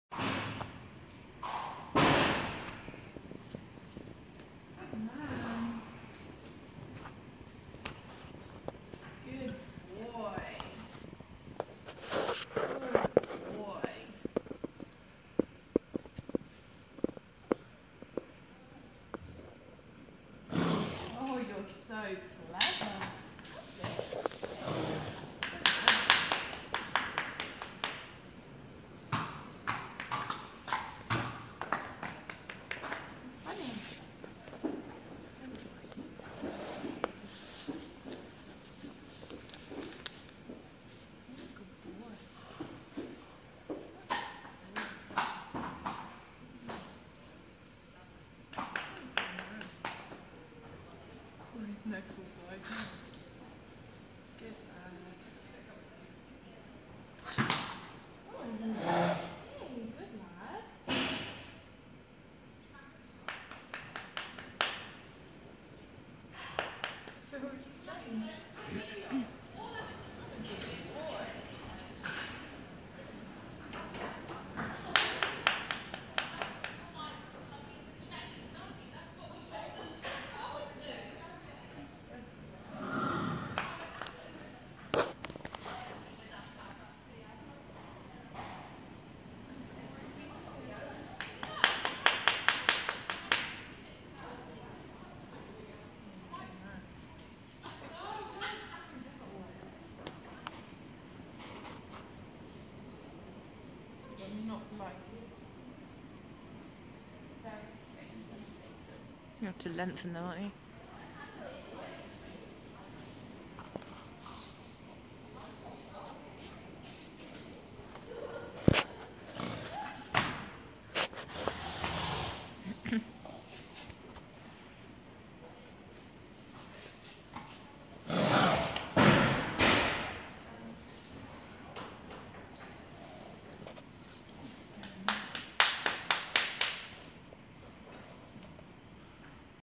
Adam Equilibrium Massage Pad first time. Post kissing spine op
Demonstrating what a brave lad Adam is! 13 months post op.